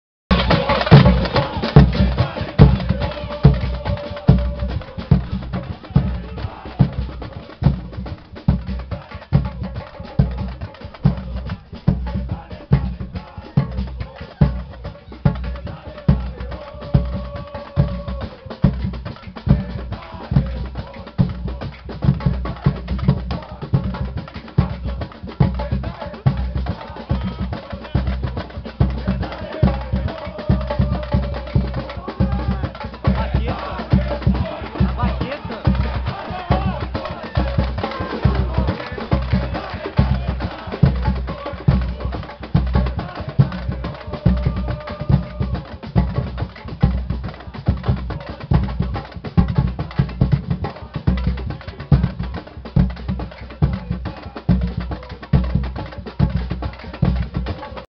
Fúria Alvi Azul Bateria
Bateria da Fúria Alvi Azul no jogo contra o glória de vacaria.